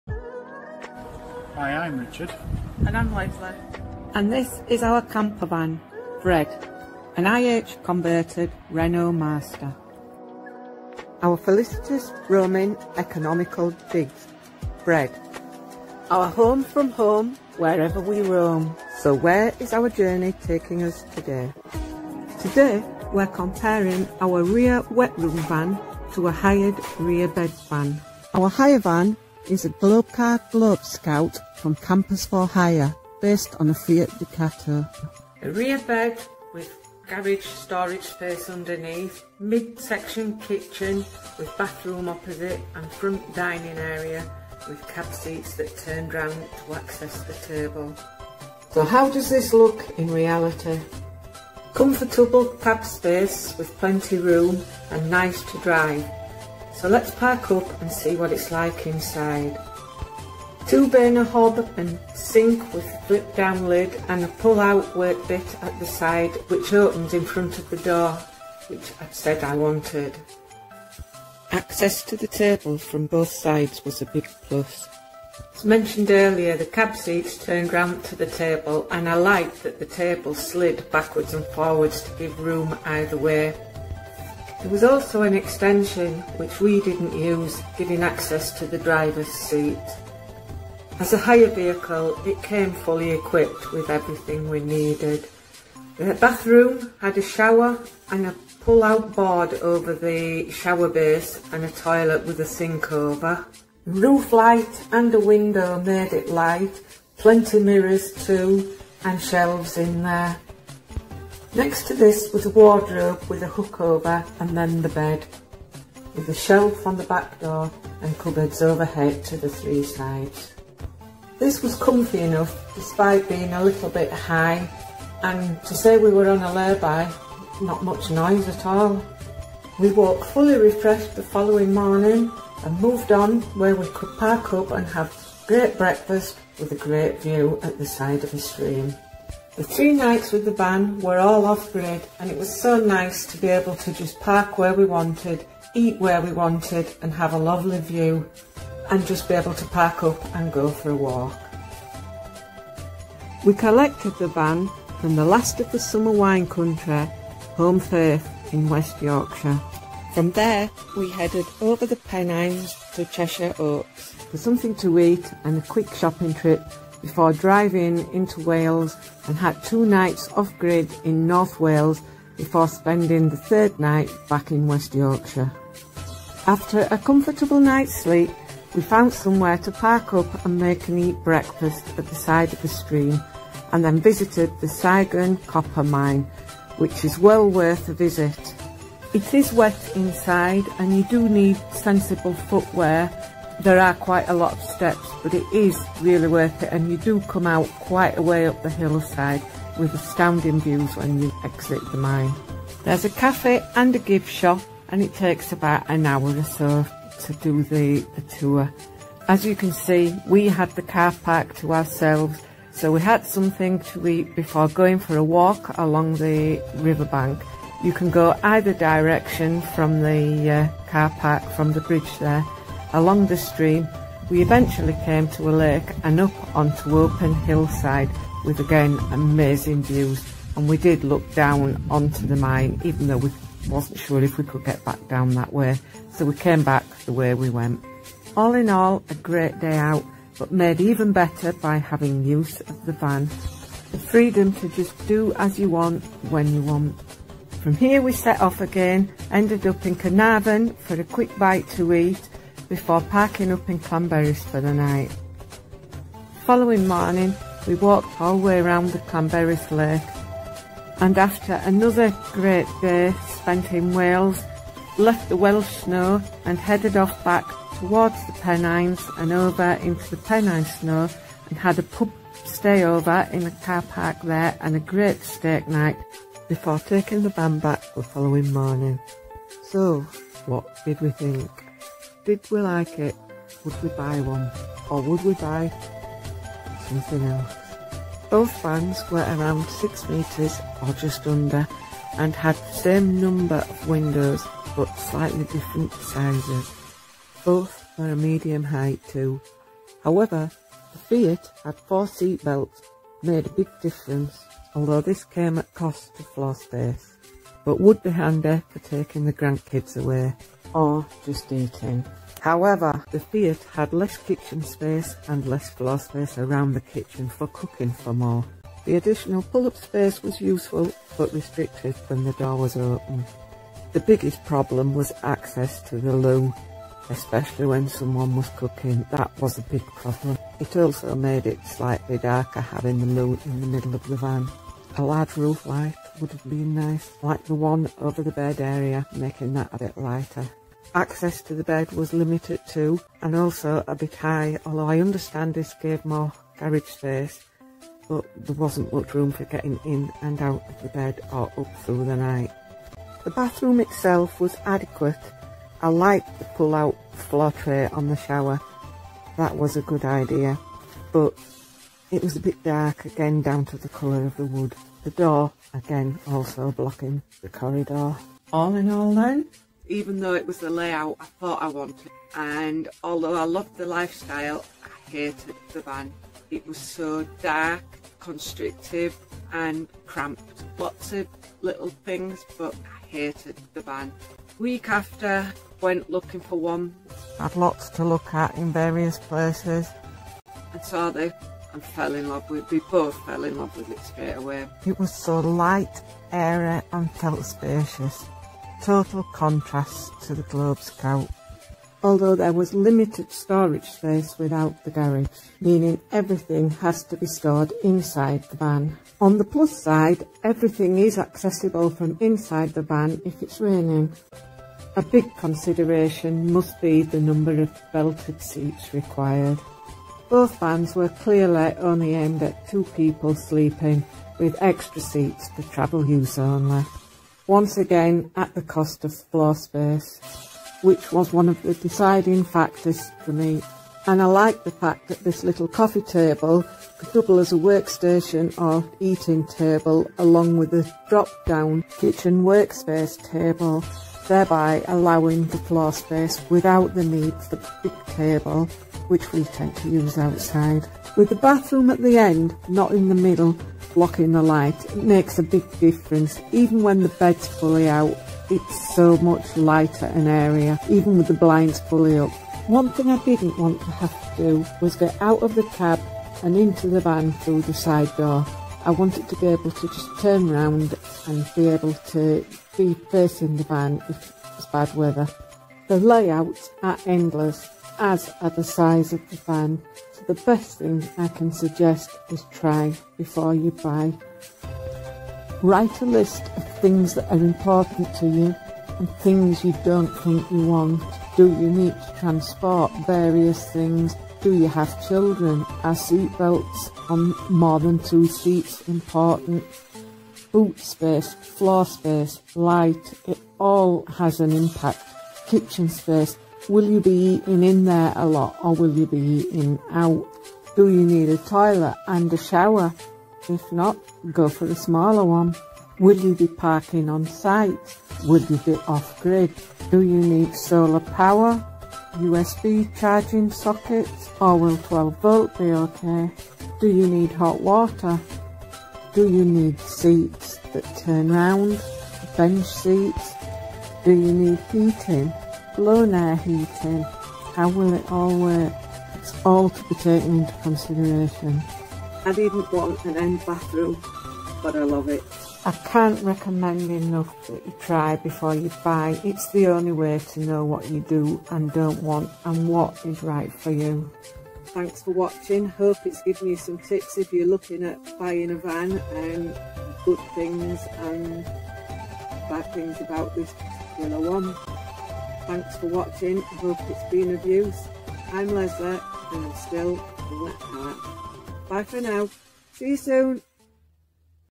Comparing a rear bed hired campervan to a rear wet room van
Comparing a hired rear bed van to a rear wetroom van including good & bad points of each

Both vans were professionally converted from new but information is relevant to DIY too

The rear bed was a Globecar Globescout Fiat Ducato
The rear wetroom an ih Oregan Renault Master

Both vans are medium height & length 6m
Both had swivel captain seats, wall mounted TV, fixed TV aerial, pull out awning, full size 3 way fridge, dual heating (propane or 240v when on ehu) trigger controlled showers & cassette toilet etc

The Ducato only had a 2 burner hob while the Master had a 4 burner hob & oven with grill all propane

The Ducato had a fixed bike rack & although the Master hadn't it did have a towbar so could use a tow bar bike carrier

The Ducato had a garage with 2 open out doors, below the bed which dismantles for transporting bulky items, which was handy for storing shopping bags out of the way till parked up & time to sort

The Ducato was hired from campers4hire in February all 3 nights were off grid 2 in N Wales 1 in W Yorkshire
We ate out on 2 of the 3 evenings & cooked breakfast each morning in the van

We found the rear bed van cramped &
lacking floor space due to the central wet room
The bed was high & awkward to get into but extra garage storage below was handy
The bulkhead was obstructive

The dark wood contributed to a cramped oppressive feel generally dark & dingey

The fridge opened the wrong way for easy access & like the wet room door blocked the already restricted path down the centre of the van, which was the kitchen area so caused disruption if access to the bed/wetroom required when kitchen in use or visa versa which was annoying

The extra belted seats for daytime/occasional use was a bonus which is the biggest drawback with the Master which only had 2

Seating each side of the table was convenient but less floor space & the TV would be a problem if more than 2 people seated

OK for a few days, but not suited for longer stays and I certainly couldn't live in it

The rear wetroom with wardrobe in took up the entire back of the Master which was private & out of way

With no high structures in the middle of the van it felt light & spacious lots of floorspace with the bed doubling as an L shape sofa, although this could be a problem for making up the bed/storing bedding etc using coordinating bedding as cushions it hasn't been

There are 3 tables, a coffee table on a central post but has feet for use in or out, drop down one doubling as workspace and a std table that's stored away ideal for outdoor use

The rear wetroom van has a centralised open living space so no bulky wetroom or wardrobe etc restricting access to the kitchen area/fridge/bed etc so light & plenty room in the kitchen for cooking etc it had twice as many hobs and an oven and grill so far more practical.
Probably due to the German Globescout being designed for more outdoor cooking

The biggest issues with the rear wetroom is only 2 belted seats, which could be a problem if the van was the main transport. With an L shape sofa it could have been possible to have a front facing belted seat. Double captain seats are also available and there would be more than enough room to have one but as its only registered as a 2 seater with DVLA it's not easy to add that extra belt, despite several options available

Lack of garage space means limited storage but it does have a boot area for 2 gas bottles the spare wheel and access to the toilet cassette but with the tow bar we use a storage rack for bikes that can house a storage box and bends out of the way to still allow access to the boot space so again not been an issue.
Under seat storage has been more than sufficient even when we've had full scuba gear including cylinders with us

The more time spent in this van, the more I love it, so glad we waited till we found one we both liked, its so easy to live in.

The best advice for anyone wanting to buy a van is try before you buy. Hire one even if it's only for 3 days you will learn so much about what you want & need, but more importantly what you don't. You'll have a better idea of what to look for before you buy/design something you will spend a lot of time in so will enjoy it more

I was adamant I didn't want a rear wetroom thinking no rear vision would be a problem to manouvre, but it hasn't been an issue and its great to drive, better than a car. I love driving it & parking has not been an issue

Clothiers Arms
18 Station Rd, Stocksmoor, Huddersfield HD4 6XN